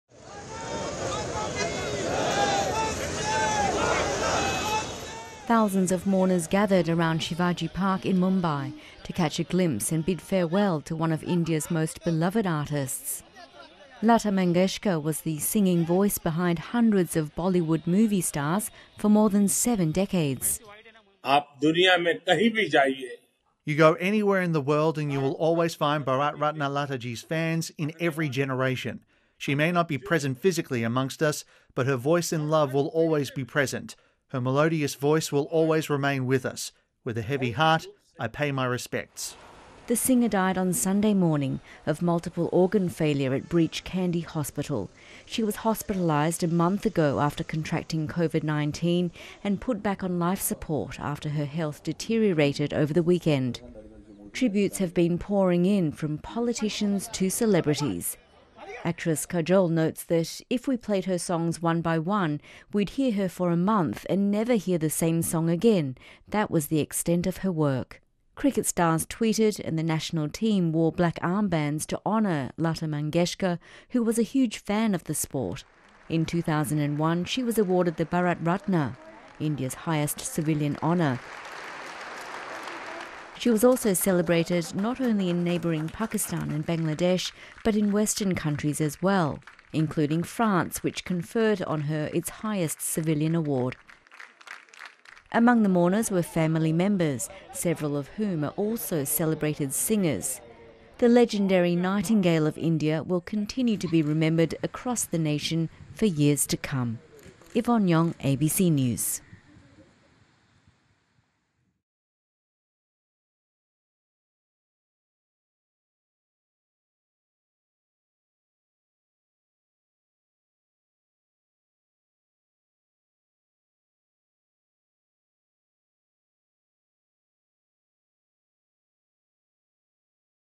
State funeral held for beloved Indian singer Lata Mangeshkar | The World
India is observing two days of mourning, after the death of one of the country's best known cultural icons. Lata Mangeshkar, dubbed Bollywood's 'nightingale', died aged 92, a month after being hospitalised for Covid-19. Prime Minister Narendra Modi joined thousands in paying their respects to the late singer at a state funeral. Yvonne Yong reports.